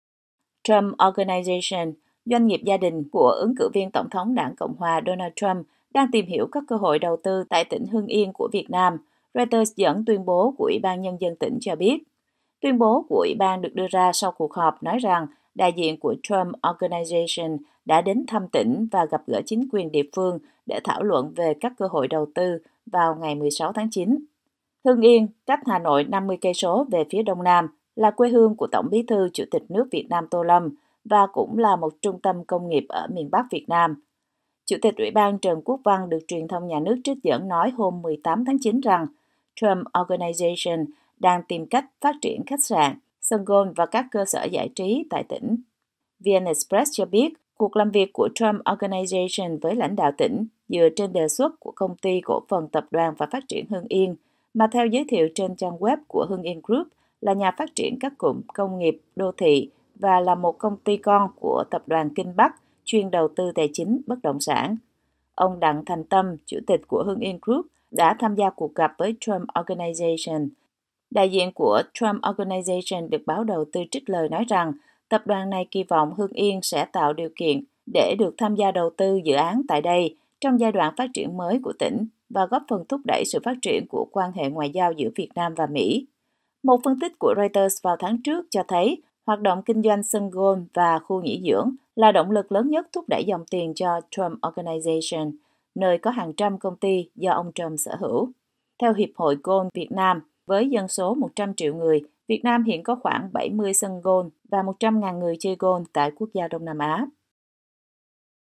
Reuters: Đại diện của Trump Organization đến thăm Hưng Yên, tìm cơ hội đầu tư | VOA Tiếng Việt
Trump Organization, doanh nghiệp gia đình của ứng cử viên tổng thống đảng Cộng hòa Mỹ Donald Trump, đang tìm hiểu các cơ hội đầu tư tại tỉnh Hưng Yên của Việt Nam, Reuters dẫn tuyên bố của Ủy ban Nhân dân tỉnh cho biết.

Tuyên bố của ủy ban đưa ra sau cuộc họp nói rằng đại diện của Trump Organization đã đến thăm tỉnh và gặp gỡ chính quyền địa phương để thảo luận về các cơ hội đầu tư hôm 16/9.

Hưng Yên, cách Hà Nội 50 km về phía đông nam, là quê hương của Tổng bí thư-Chủ tịch nước Việt Nam Tô Lâm và cũng là một trung tâm công nghiệp ở miền bắc Việt Nam.

Chủ tịch ủy ban Trần Quốc Văn được truyền thông nhà nước trích dẫn nói hôm 18/9 rằng Trump Organization đang tìm cách phát triển khách sạn, sân golf và các cơ sở giải trí tại tỉnh.

VnExpress cho biết cuộc làm việc của Trump Organization với lãnh đạo tỉnh dựa trên đề xuất của Công ty cổ phần Tập đoàn và Phát triển Hưng Yên,  mà theo giới thiệu trên trang web của Hung Yen Group, là nhà phát triển các cụm công nghiệp, đô thị và là một công ty con của Tập đoàn Kinh Bắc chuyên đầu tư tài chính, bất động sản.

Ông Đặng Thành Tâm, chủ tịch của Hung Yen Group, đã tham gia cuộc gặp với Trump Organization, vẫn theo VnExpress.

Đại diện Trump Organization được Báo Đầu Tư trích lời nói rằng tập đoàn này kỳ vọng Hưng Yên tạo điều kiện để được tham gia đầu tư dự án tại đây trong giai đoạn phát triển mới của tỉnh và góp phần thúc đẩy sự phát triển quan hệ ngoại giao giữa Việt Nam và Mỹ.

Một phân tích của Reuters vào tháng trước cho thấy hoạt động kinh doanh sân golf và khu nghỉ dưỡng là động lực lớn nhất thúc đẩy dòng tiền cho Trump Organization, nơi có hàng trăm công ty do ông Trump sở hữu.
Theo Hiệp hội Golf Việt Nam, với dân số 100 triệu người, Việt Nam hiện có khoảng 70 sân golf và 100.000 người chơi golf tại quốc gia Đông Nam Á.